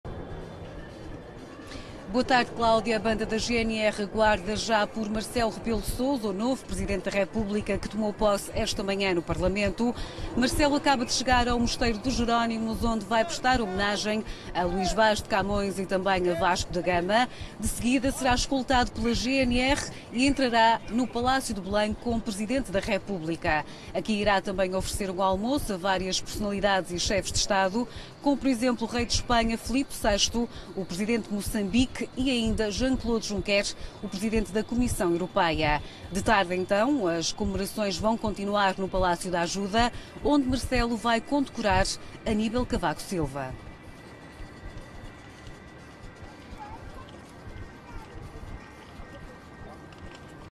Novo Presidente da República em Belém
PIVÔ
Marcelo Rebelo de Sousa tomou posse esta 4f (09/03/2016) como Presidente da República no Parlamento. De seguida prestou homenagem a Luís Vaz de Camões e Vasco da Gama no Mosteiro de Jerónimos para depois entrar pela primeira vez no Palácio de Belém como Chefe de Estado, sucedendo assim a Cavaco Silva.

Porto Canal - deleg. Lisboa
Jornalista: Salomé Pinto
Imagem: Pedro Martins